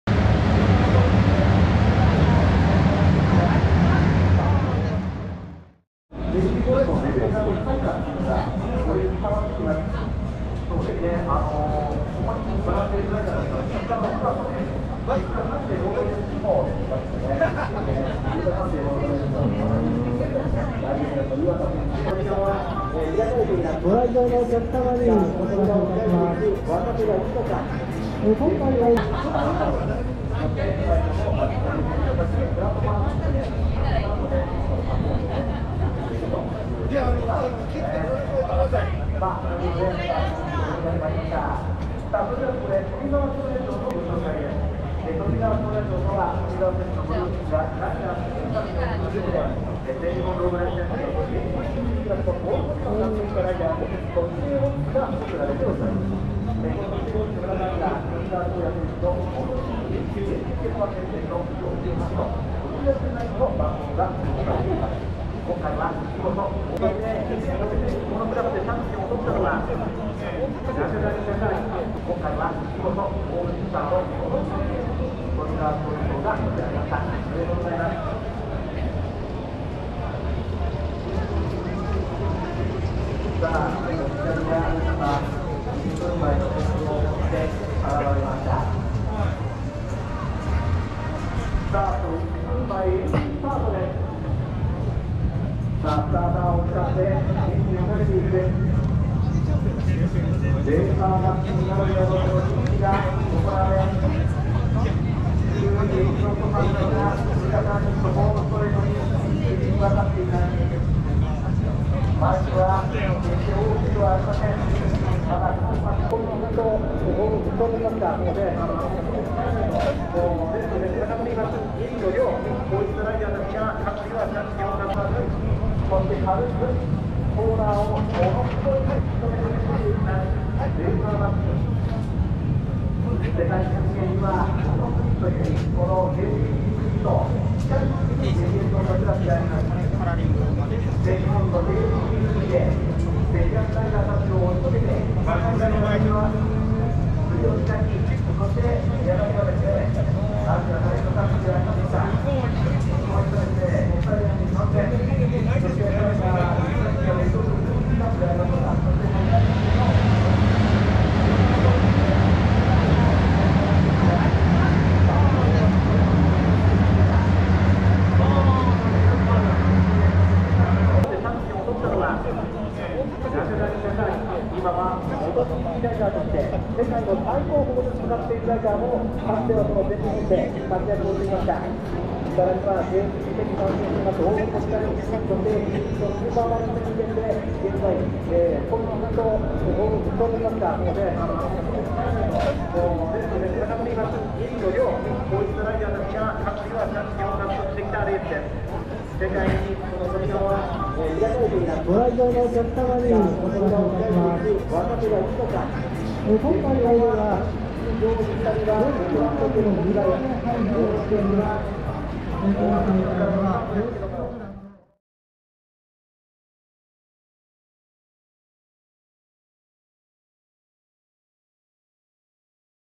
【YZF-R7】鈴鹿サーキットで話題のR7の実車を発見！ 折角なのでMT-07と比較しながら解説してみました 発売が楽しみ過ぎる１台！！ 2021MFJ-GP
鈴鹿に行ったら話題のR7が展示してあったので思わず撮影してしまいました！
コイツは文句無しでカッコイイ！！